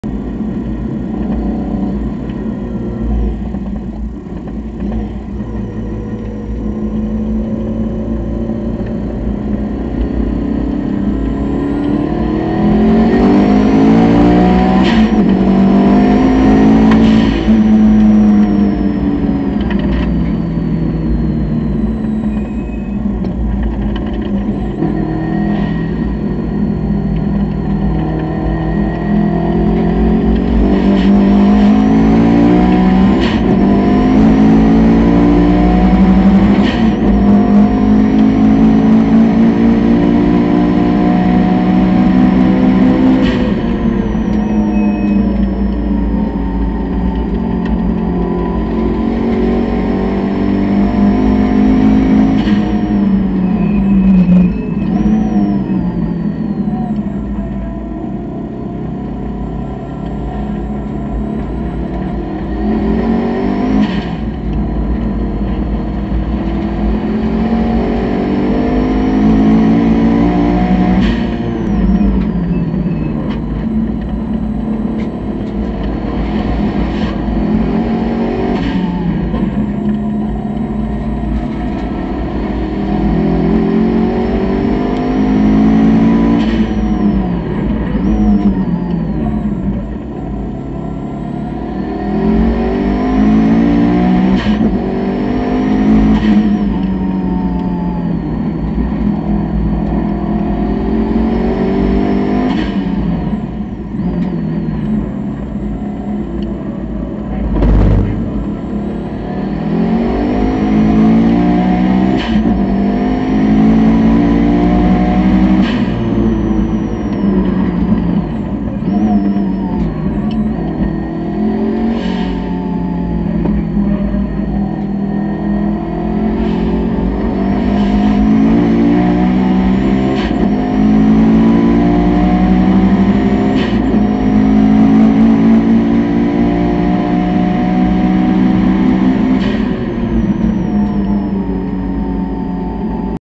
CAMS Nationals Autotech STi v V8 Supercar - 1.36.8.avi
Saturday's first session of the CAMS National Super Sprint Championship. Held at Eastern Creek. Autotech STi driven by Jason Wright chasing ex Stone Bros Racing V8 Super Car driven By Stuart Inwood